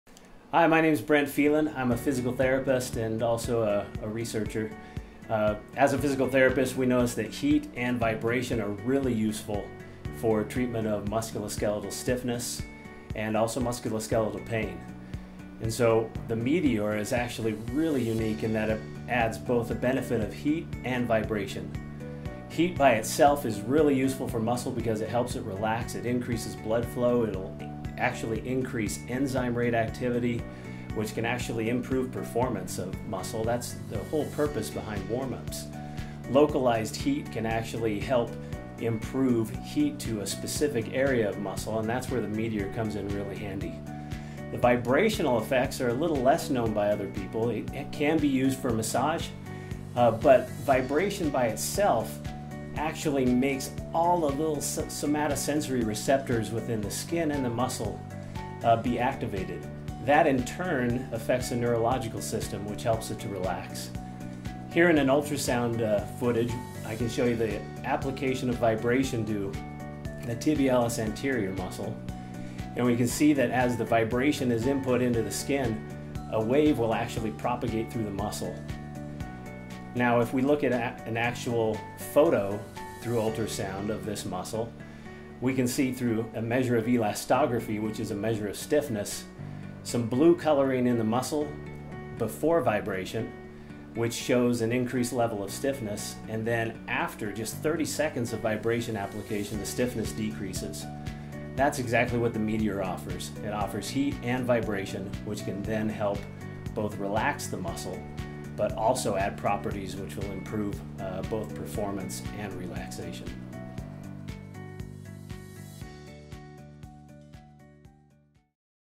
Brent Feland - The Science Behind the Meteor
Brent Feland explains the science behind the vibration and heat utilized by the Meteor, MyoStorm's flagship muscular recovery product.